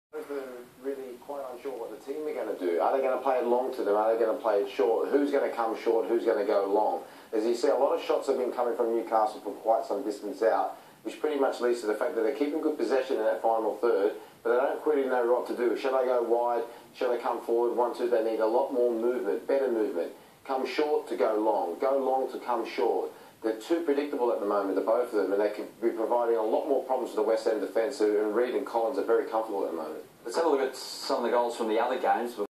Classic punditry from Bosnich
Adam and Robbie bemused by the chat of bozzer